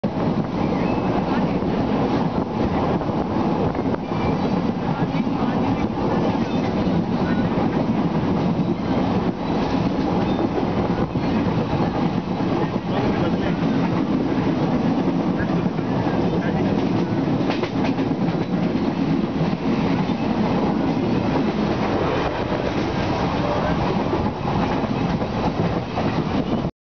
Crossing NH-56
Lucknow bound 14215 up Ganga Gomti Express Crosses the Varanasi-Lucknow Road (NH-56) at Arjunganj crossing